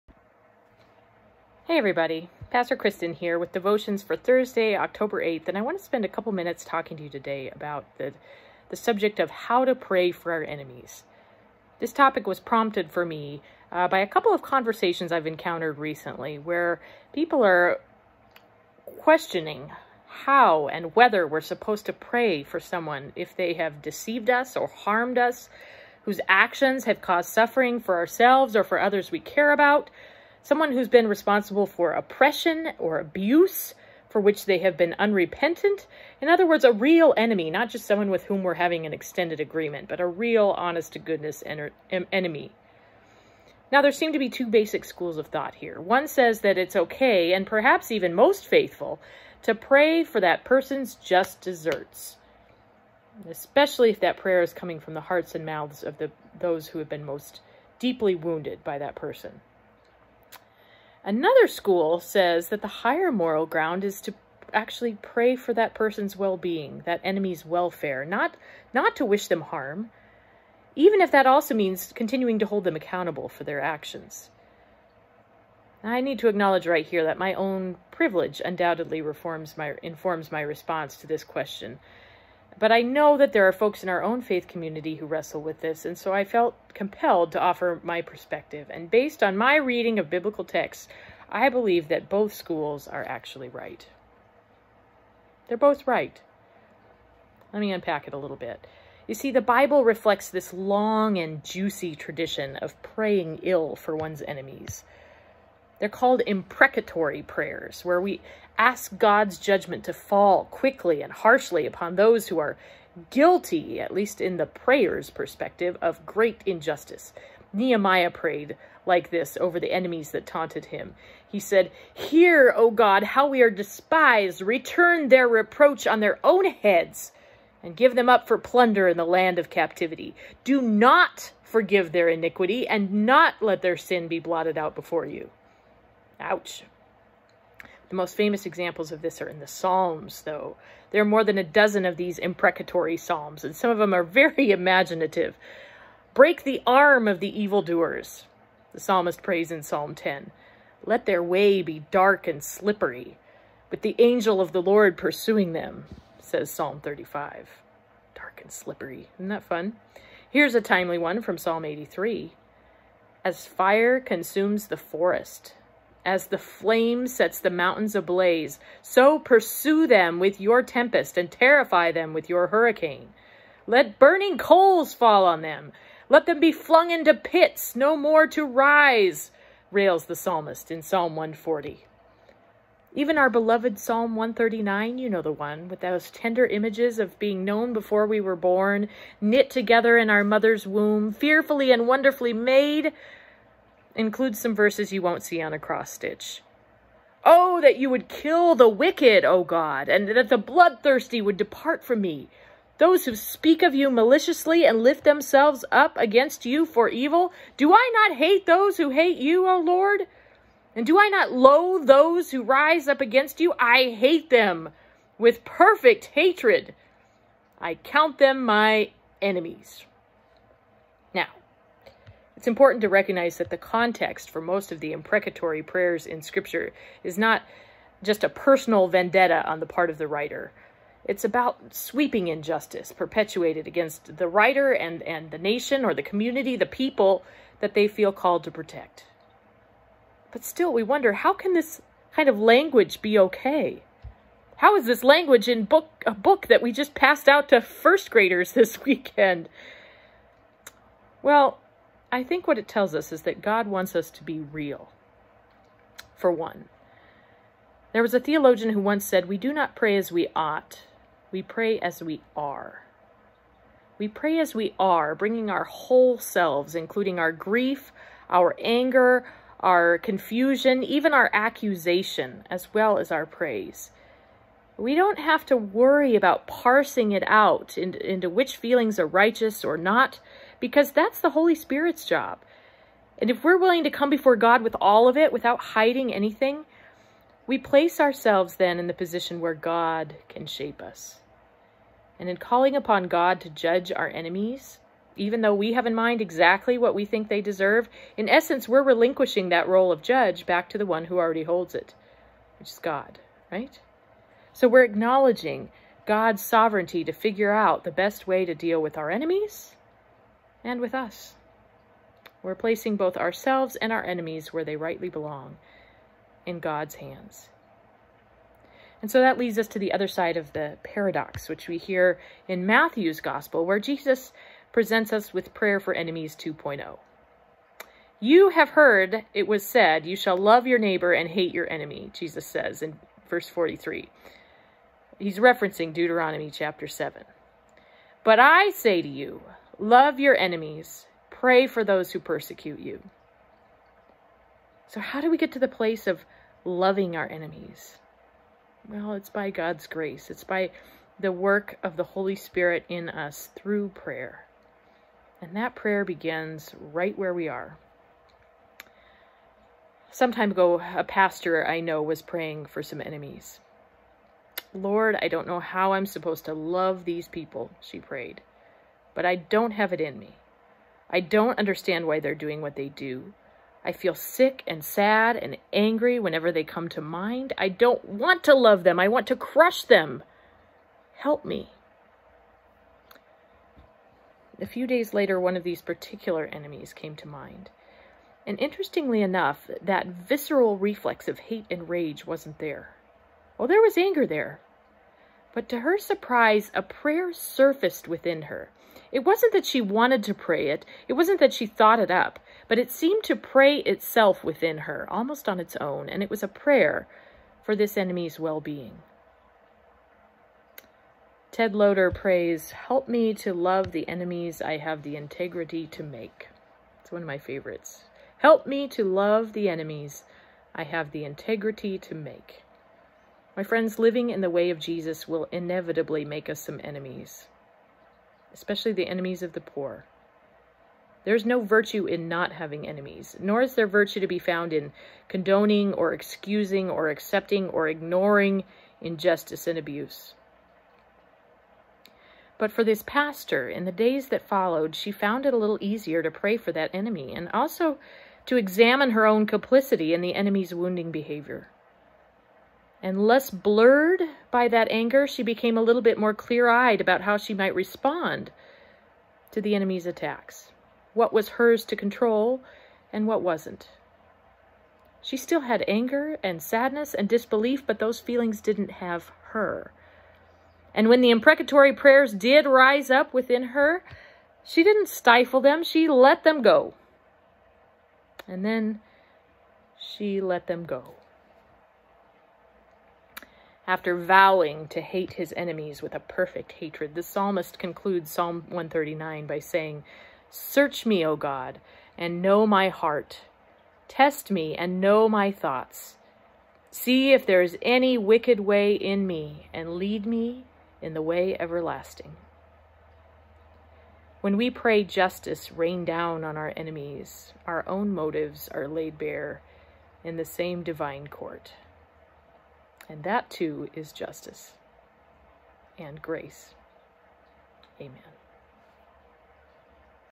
Daily Devotions October 8, 2020
Join Pastor Kristin Rice for today's devotion.